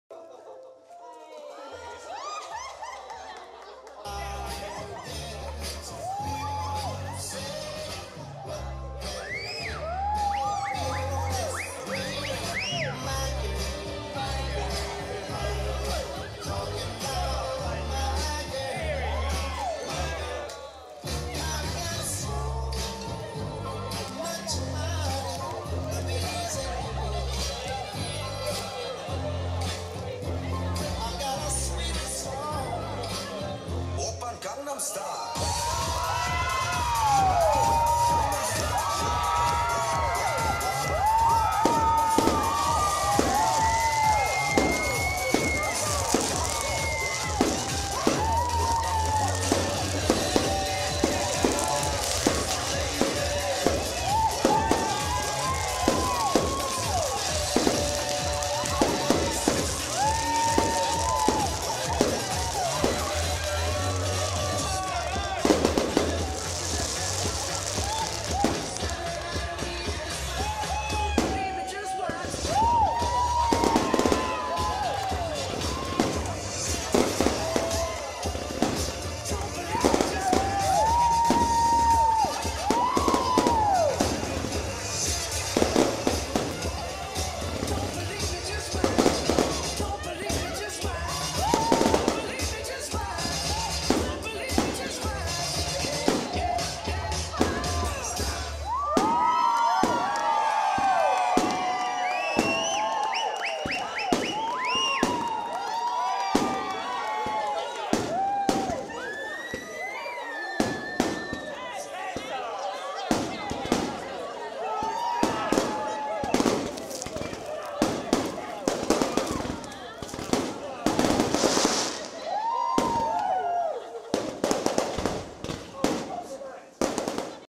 Best Surprise Father Daughter Dance Ever!!! *Funny [ Santorini, Greece] by Phosart.
Like father, like daughter!!
“Dad. He can play like a kid, give advice like a friend and protect like a bodyguard.”
And if the music is good, you dance :)

*Daddy cool moves came out in the rhythm of the music. Daughter and father gave us something special to remember and all that in the most magical island of the world under the lights of huge fireworks!!!